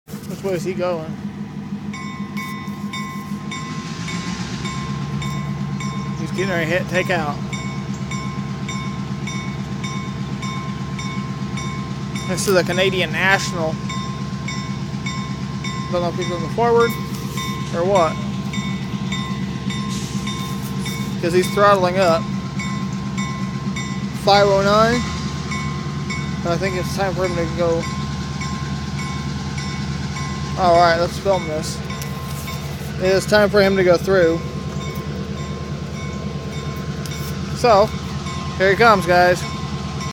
CANADIAN NATIONAL 9638 FREIGHT TRAIN PREPARES TO TAKE OUT
THIS WAS FILMED THIS EVENING AT 5:09 PM CDT AT CANADIAN NATIONALS TRACK LOCATED ON SOUTH 4TH STREET IN EFFINGHAM ILLINOIS